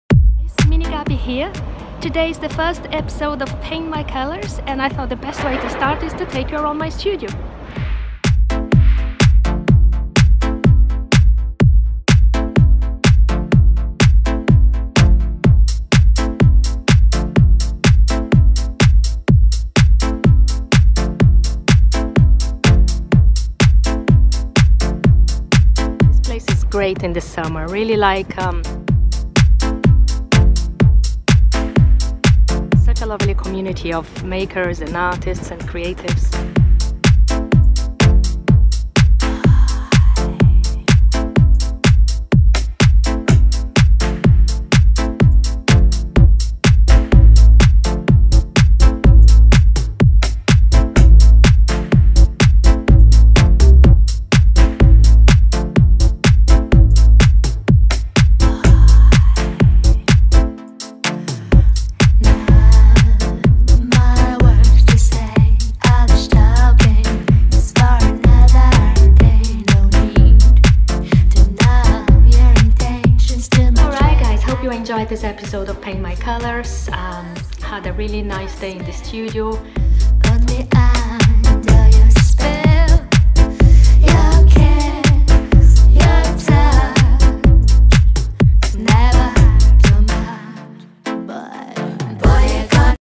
Artvlog, a tour to my studio- Paint My Colours ep.1
First episode of Paint My Colours - I take you around my studio in London ..

you can find out more about my art on my portfolio